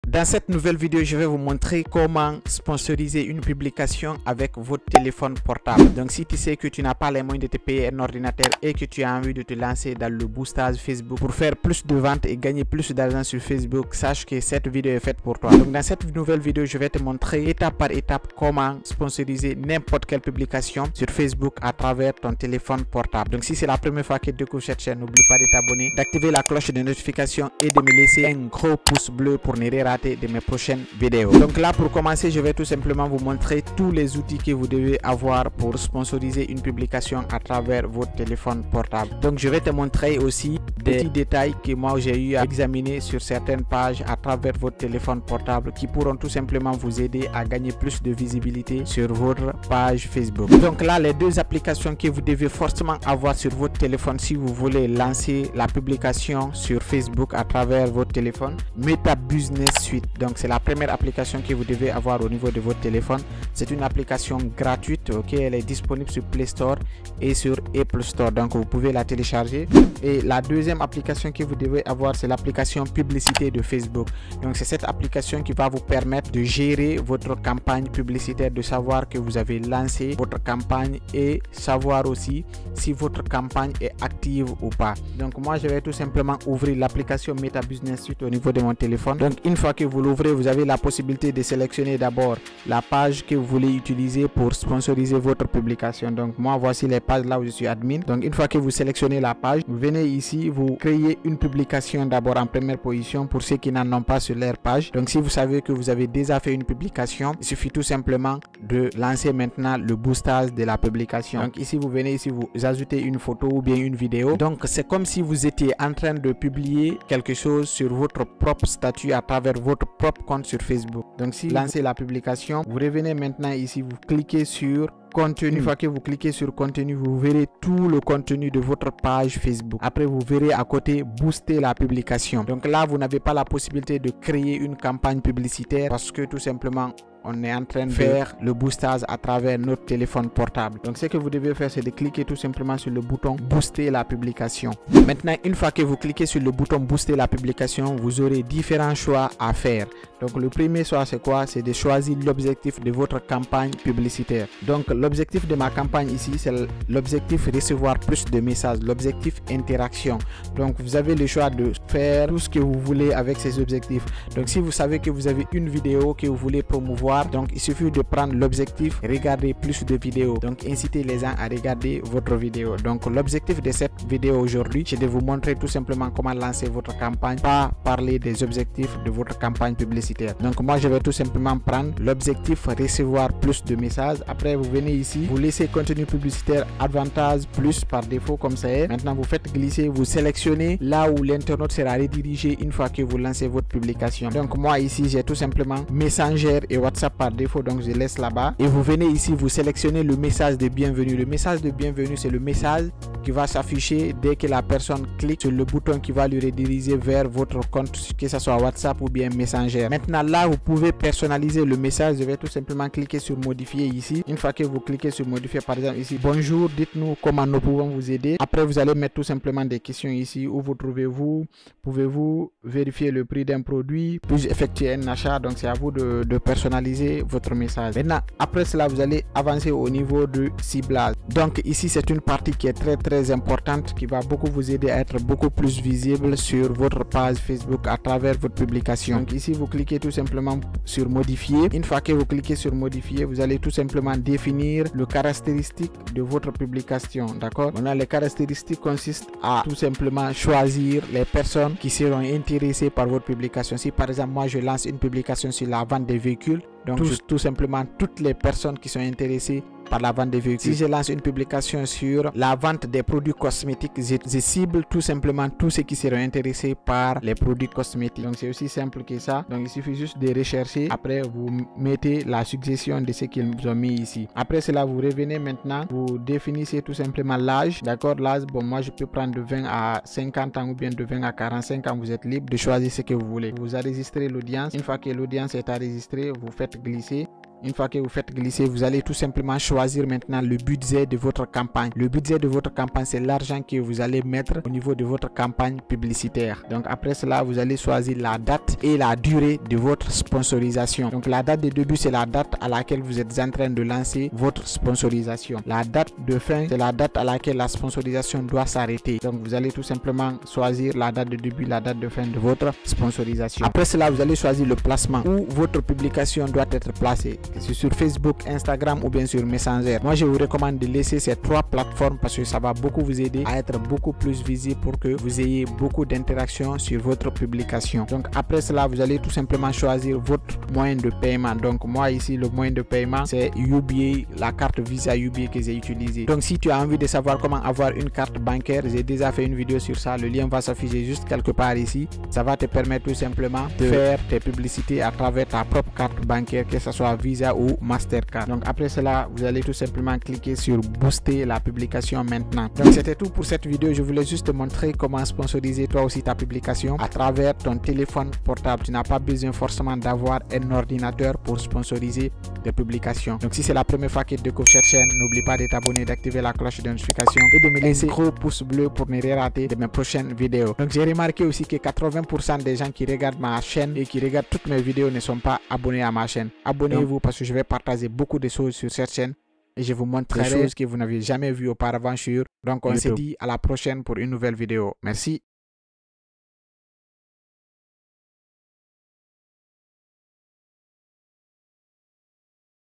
Comment Booster une Page Facebook avec son Téléphone En 2025
Comment booster une publication sur une page Facebook avec son téléphone ? C'est ce que nous allons voir dans cette vidéo. Comment booster une publication en ligne avec son téléphone portable ? Comment publier une photo avec son téléphone portable ? Toutes les techniques sont ici ! Si vous aussi, vous souhaitez booster votre page Facebook, cette vidéo est faite pour vous !